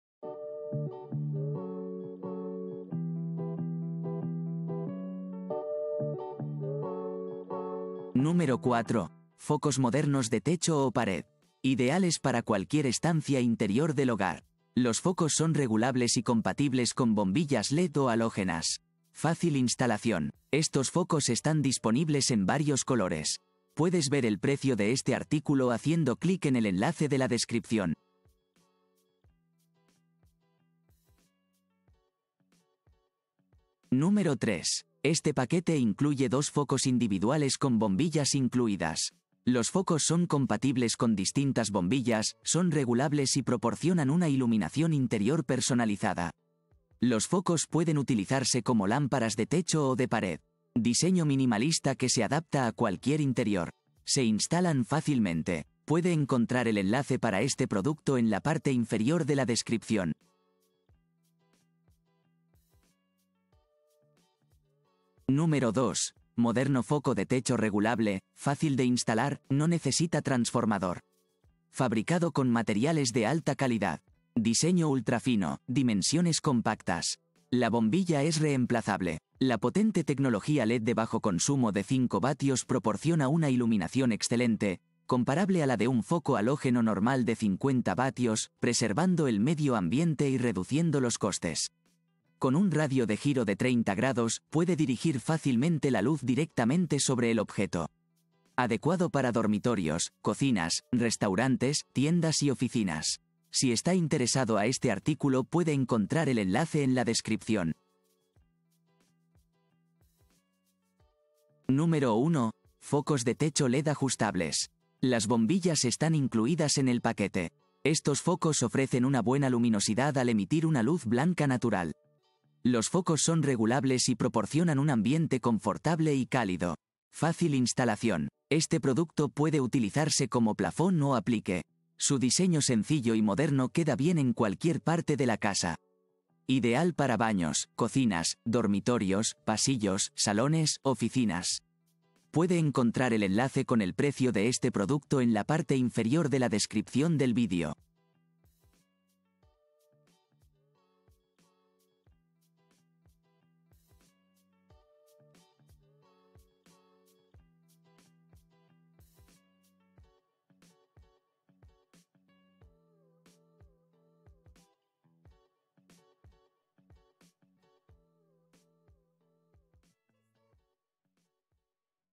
Los 4 mejores focos de techo orientables
1) Bojim Lámpara de Techo LE... » Amazon
2) KYOTECH Focos LED Techo F... » Amazon
3) Bojim 2 Pcs Focos LED Int... » Amazon
4) Ketom GU10 Lámpara de Tec... » Amazon

✋ Aquí están los 4 mejores focos de techo orientables, disponible en el mercado.

00:00 - Intro
00:08 - 4) Ketom GU10 Lámpara de Techo con 3 Focos, Focos de Techo Orientables, Blanco Apli...
00:48 - 3) Bojim 2 Pcs Focos LED Interior Techo, Aplique de Pared Giratorios y Orientables,...
01:34 - 2) KYOTECH Focos LED Techo Focos Superficie Giratorio incl. Bombilla 5W intercambia...
02:06 - 1) Bojim Lámpara de Techo LED 2 Focos, Focos LED Interior Techo Giratorios y Orient...